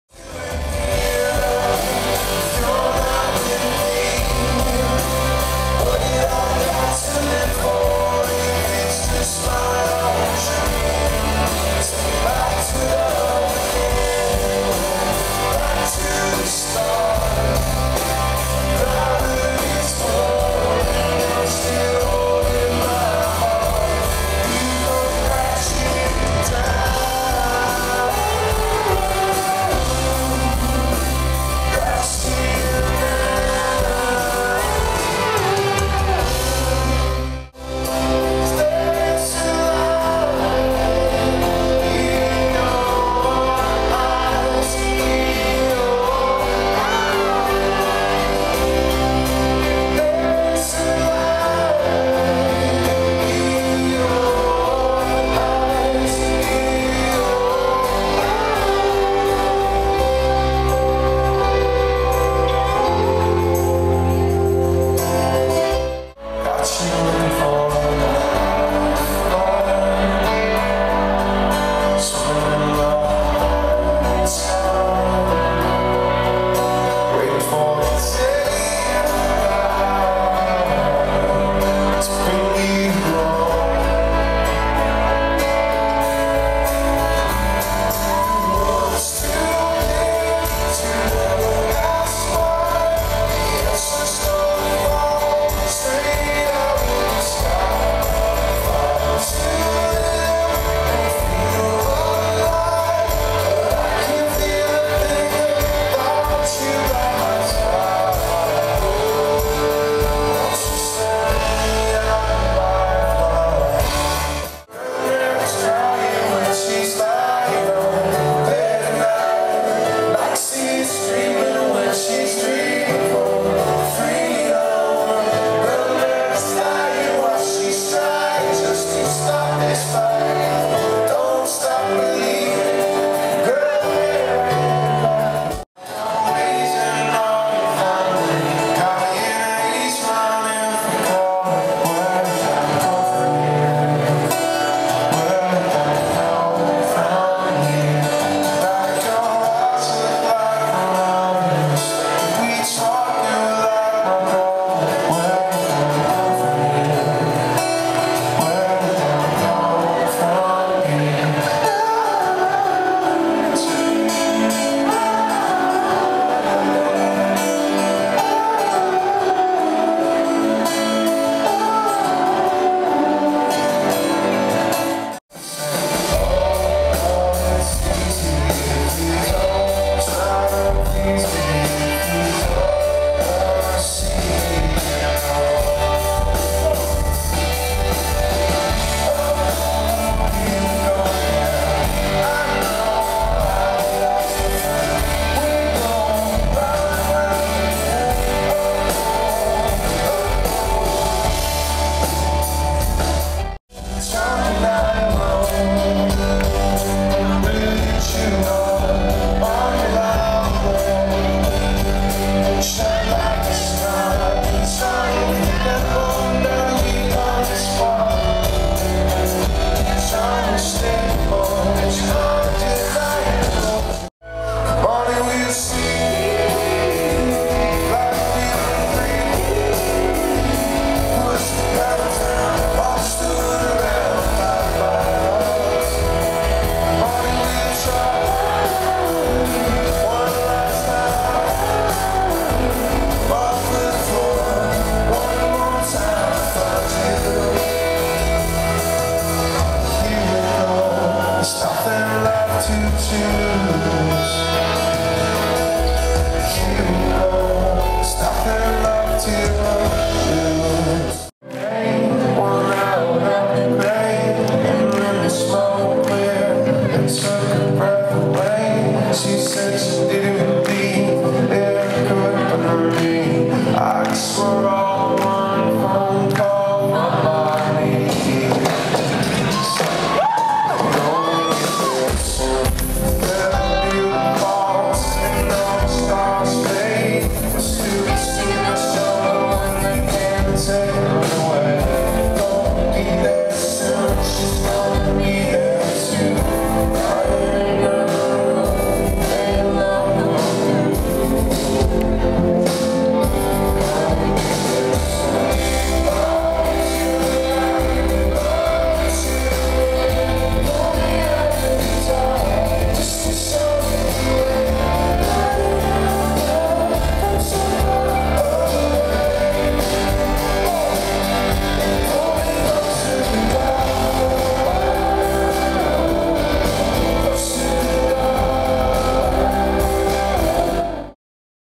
Mat Kearney - 9/23/09 - House of Blues Boston
Mat Kearney Concert on the 23th of September 2009 at the House of Blues in Boston